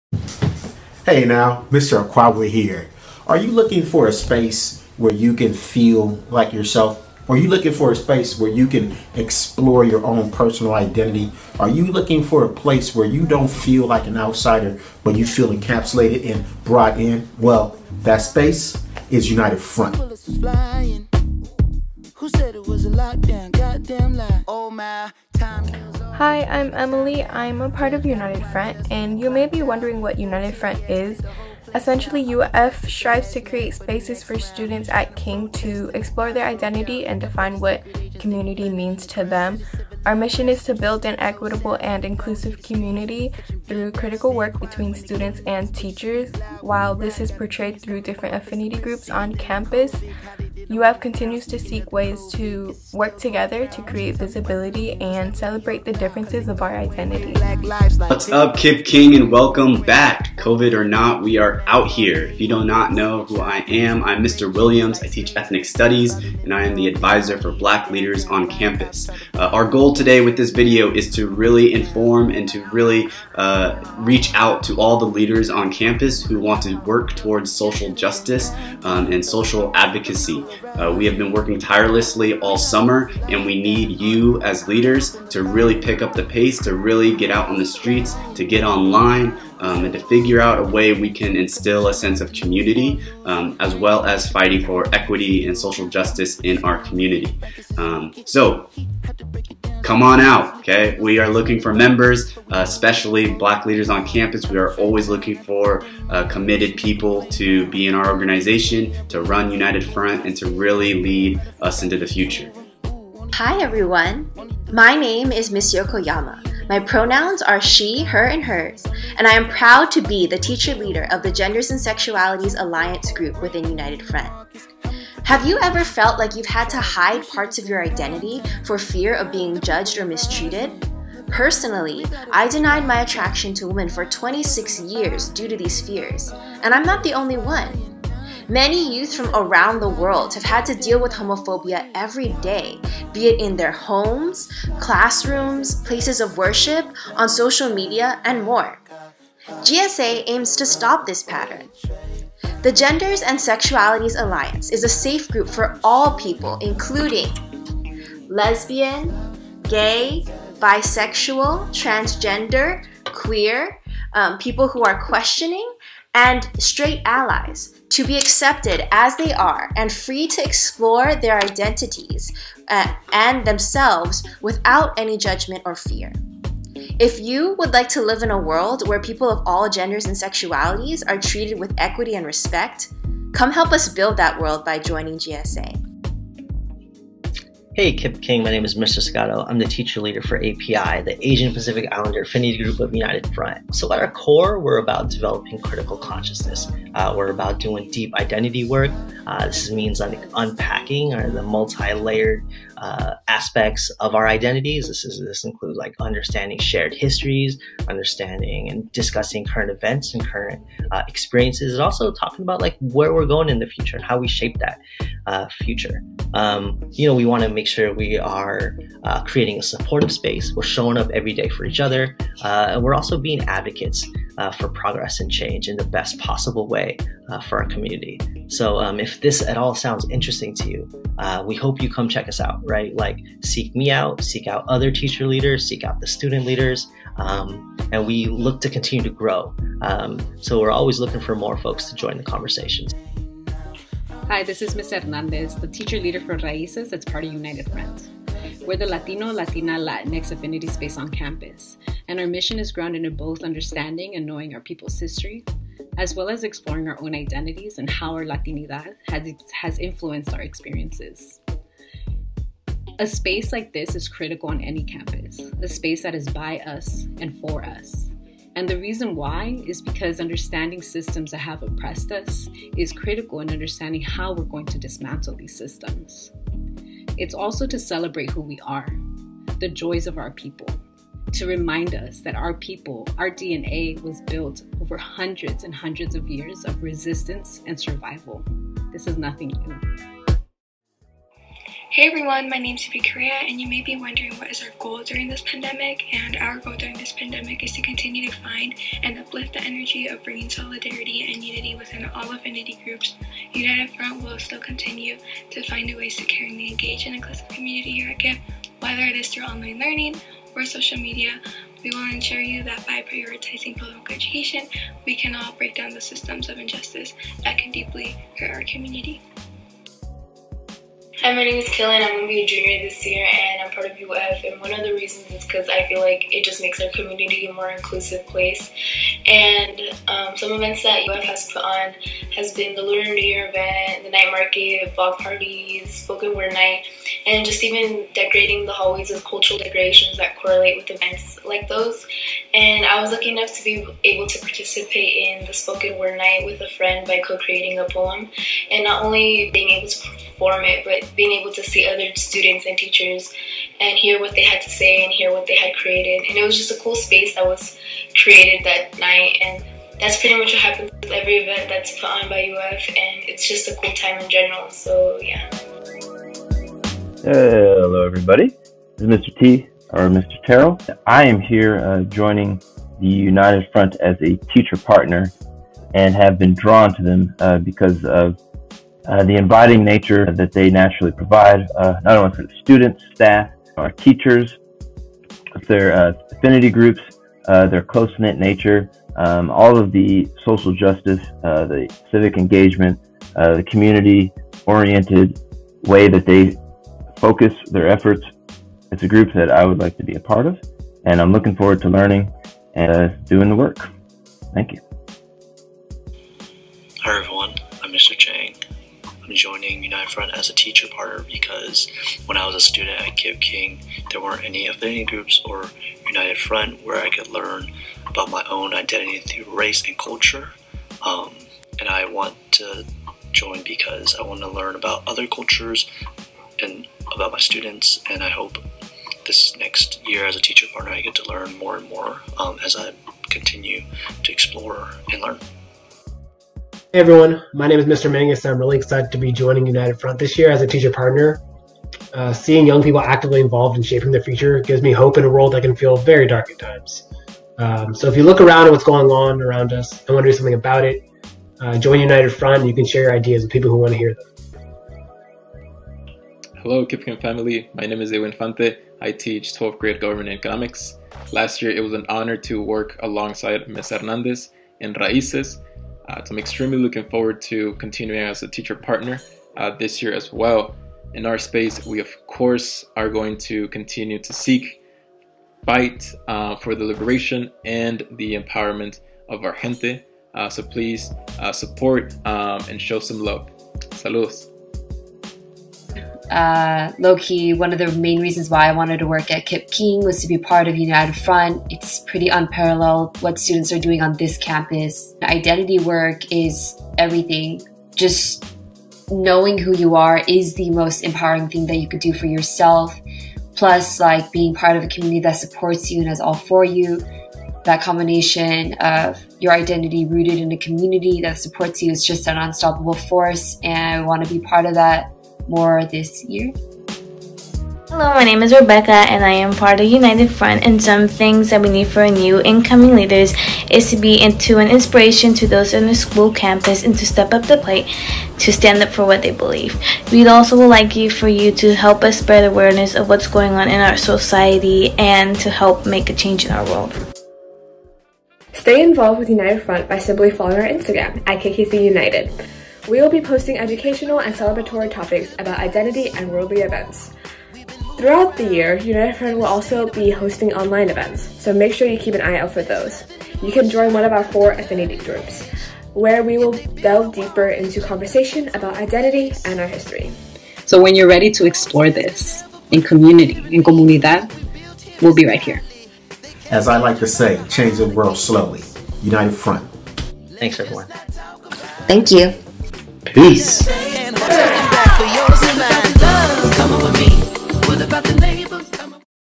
United Front 2020-2021
This video describes United Front, an organization within the KIPP King Collegiate High School Community whose mission is to build a more equitable and inclusive community through the critical work of student and teacher leaders in affinity group spaces.